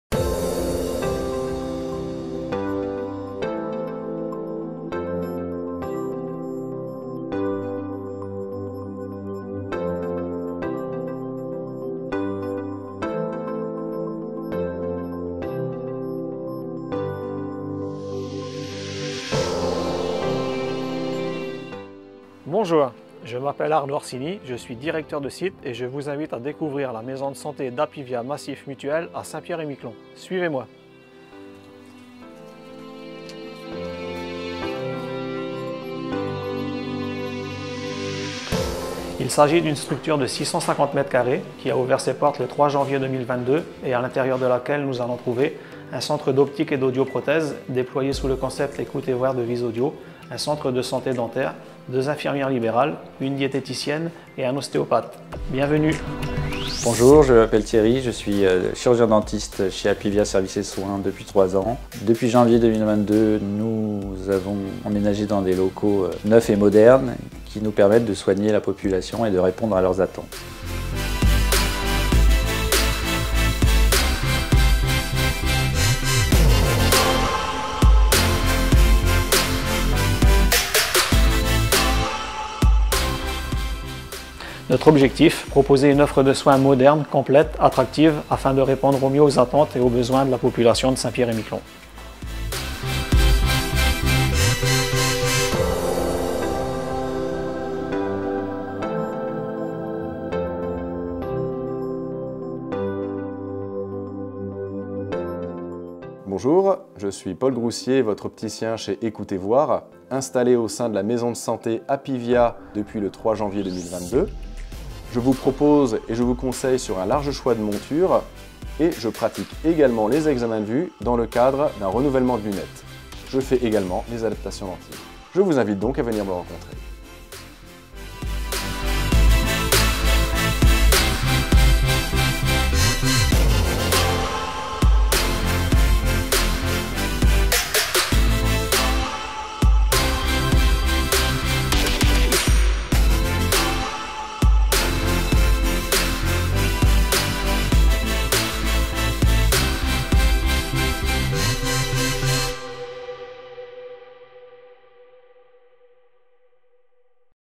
Maison de santé Apivia à Saint-Pierre-et-Miquelon
Apivia Macif Mutuelle, présente sur l'archipel de Saint-Pierre-et-Miquelon, a participé à la construction d'une Maison de santé qui a ouvert ses portes en janvier 2022. L'objectif est d'assurer un meilleur service et accès aux soins aux habitants, en leur proposant une offre complète et moderne, pour répondre aux besoins de santé de tous.

Dans cette Maison de santé, située à Saint-Pierre, les habitants de l'Île trouvent un centre d'optique et d'audioprothèse Écouter Voir, un centre de santé dentaire, un ostéopathe, une diététicienne et deux infirmières. La Maison de santé est ouverte du lundi au vendredi de 8h30 à 12h et de 13h30 à 18h. Le centre d'optique Écouter Voir est également ouvert le samedi de 9h à 12h.